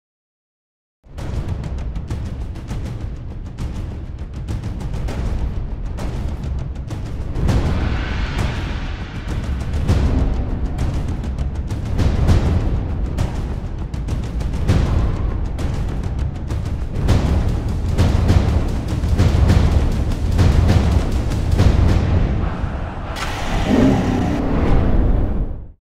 "Ignite"--Imported From Gotham City
This was my entry into the Chrysler/The Dark Knight Rises contest, but it didn't make it to the voting stage for some reason.